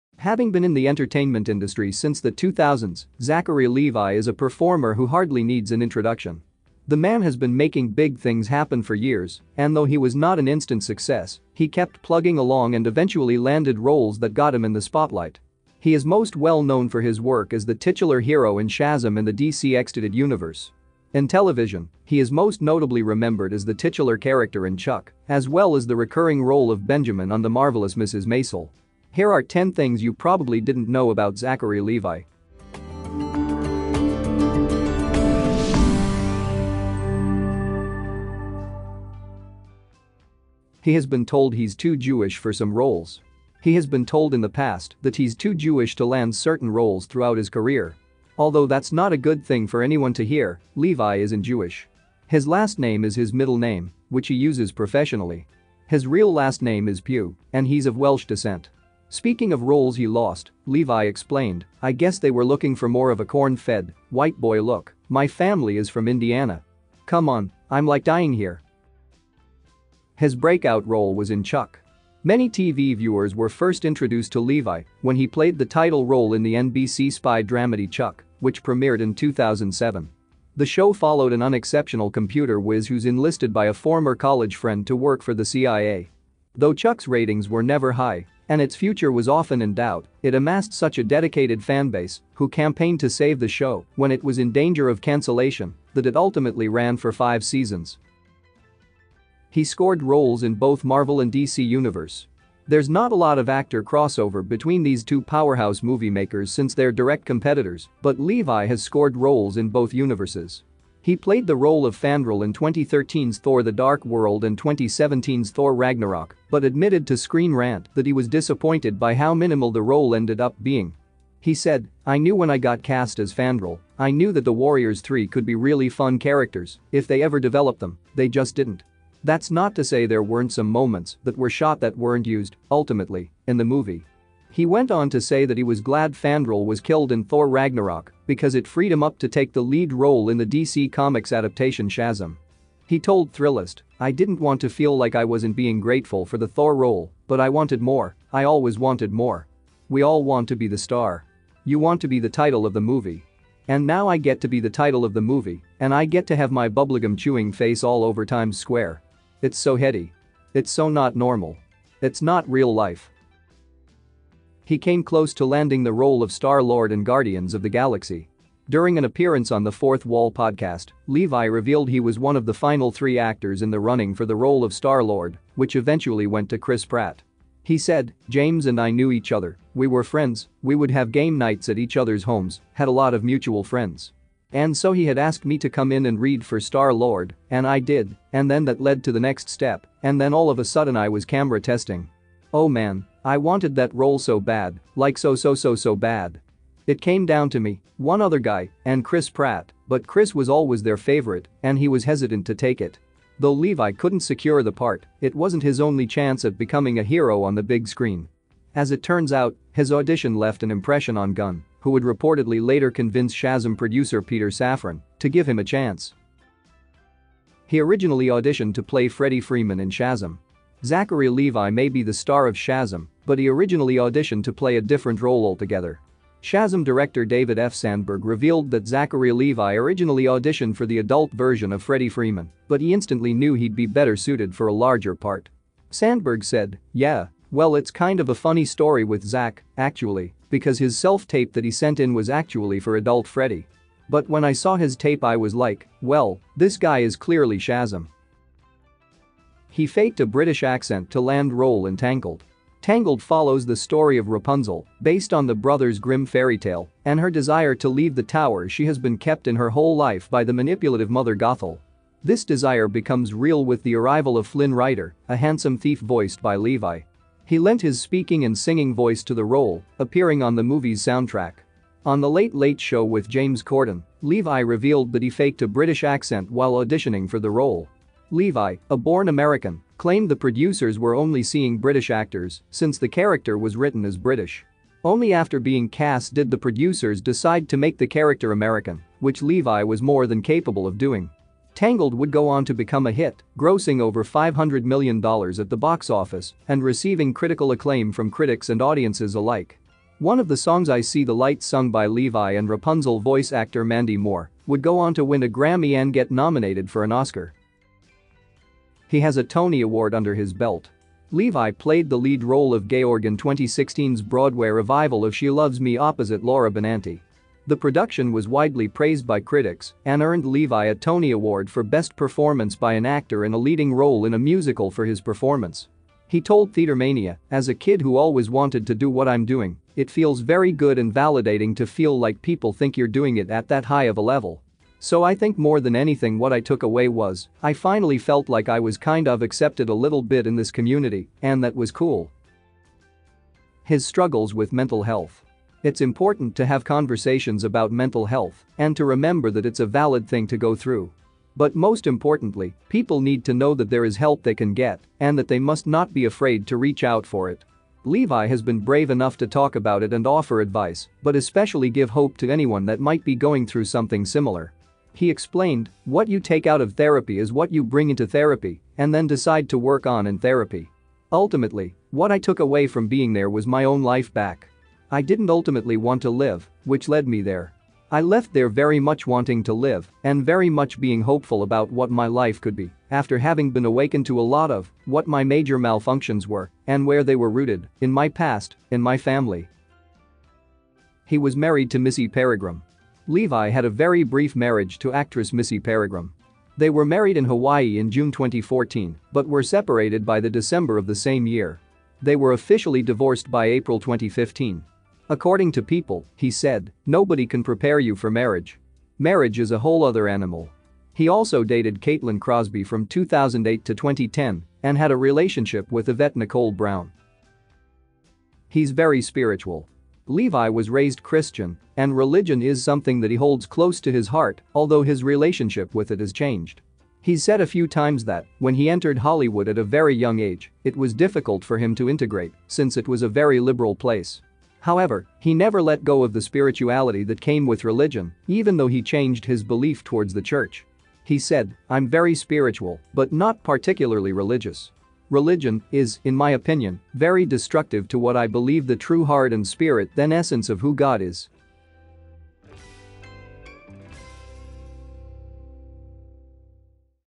10 Things You Didn't Know About Zachary Levi | Star Fun Facts
Some things you probably didn't know about Zachary Levi